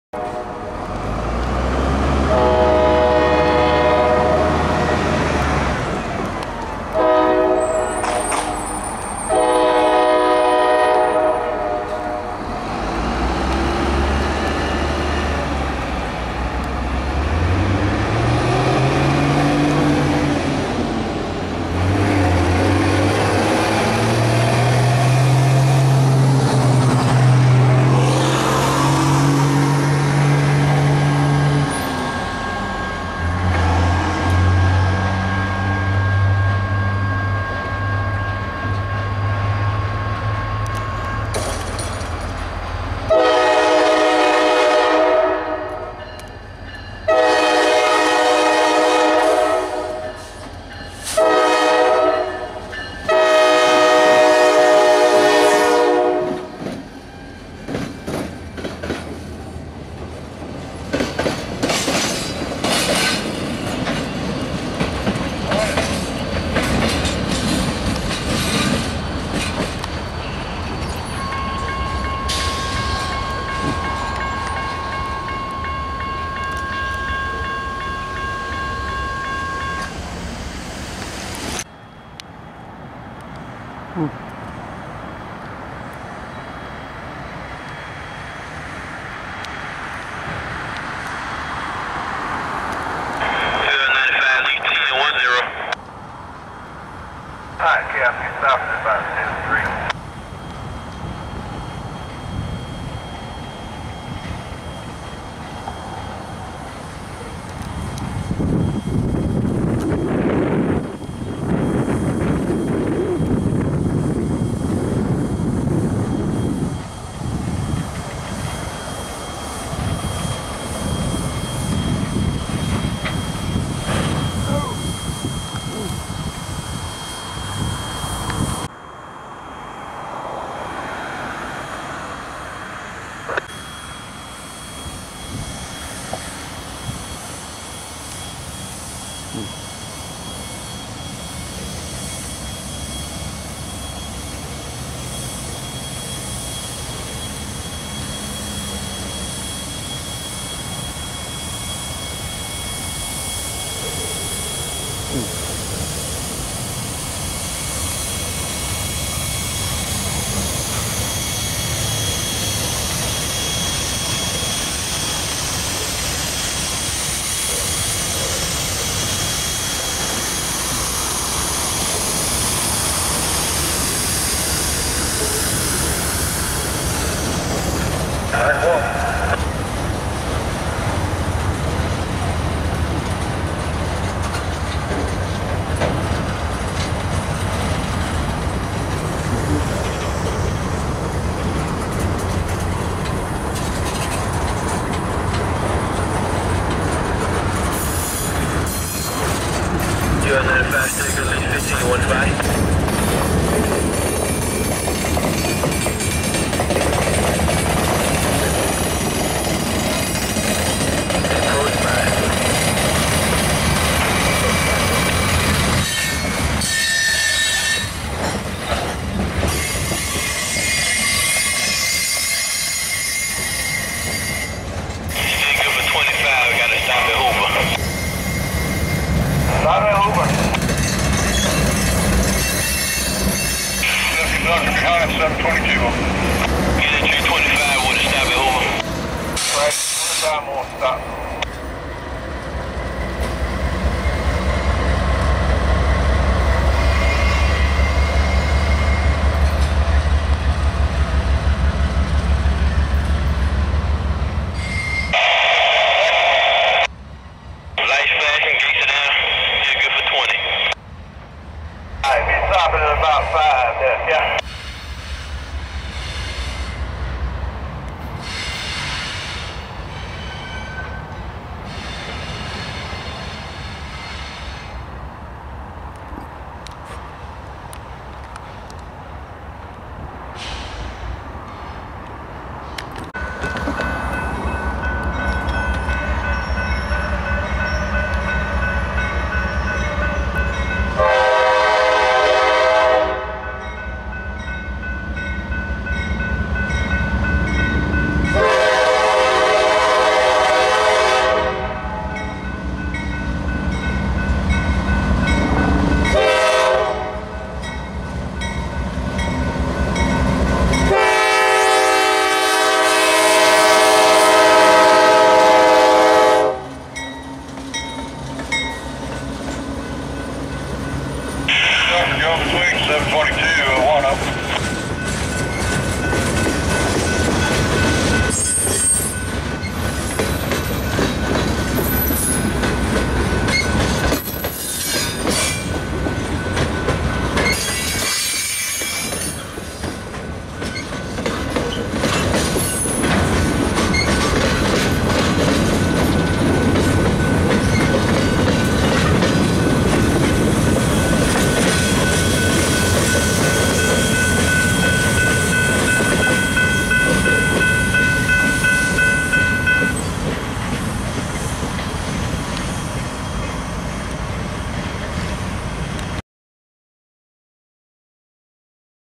Railfanning Carolina Juction
Took some time to work Portlock yard and catch a couple of Locals on the move. Here is NS 732 (EMD RPE4C) and NS 3094 (EMD GP40-2) leading NS NV01 and CSXT 384 (GE AC44CW)  performing a reverse move at Carolina Junction on 12/20/2017 at 10:54 am.